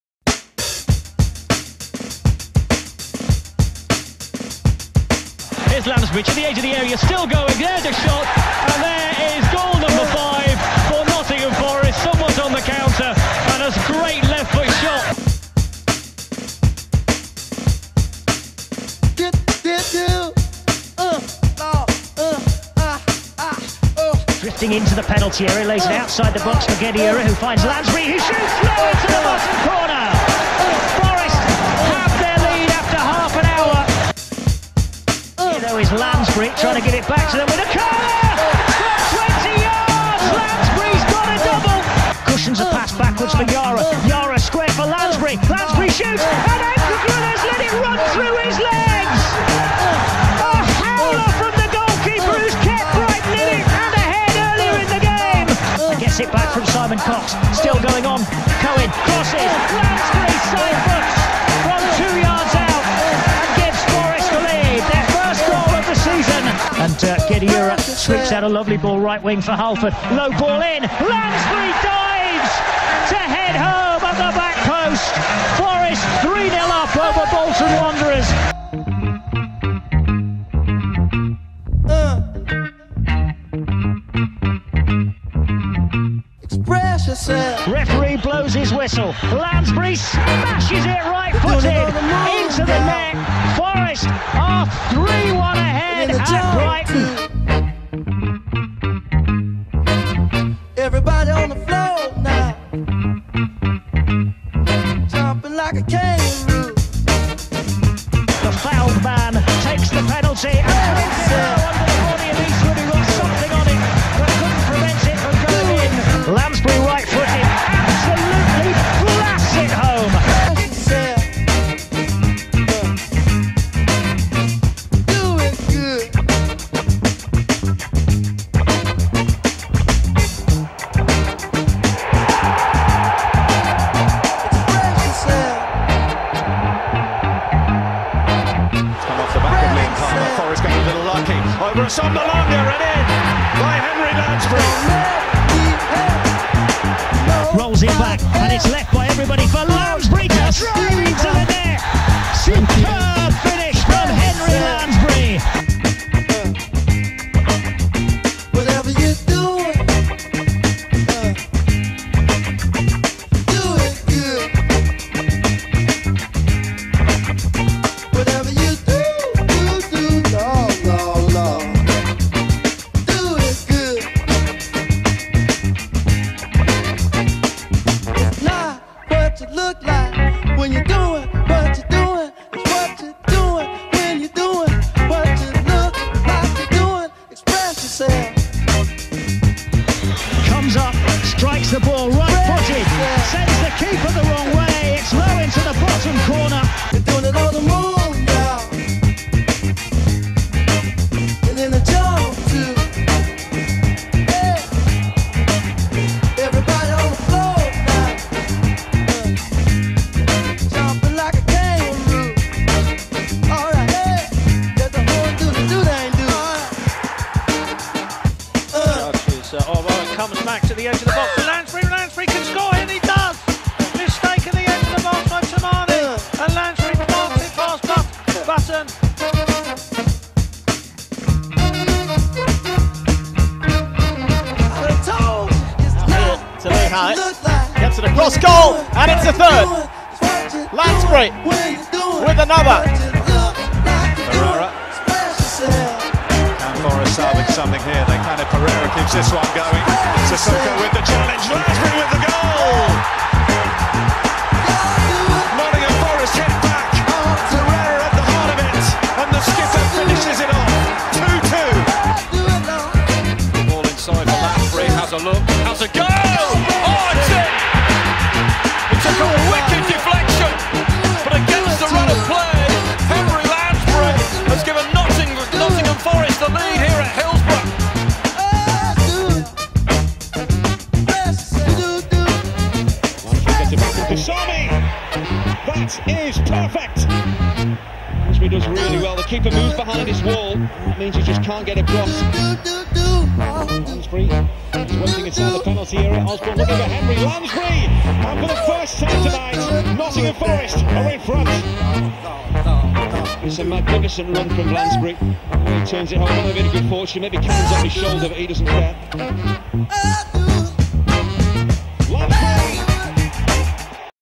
Henri Lansbury Nottingham Forest 2012-2016
Versatile midfielder Henri Lansbury amassed 149 appearances from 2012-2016 and scored 33 great goals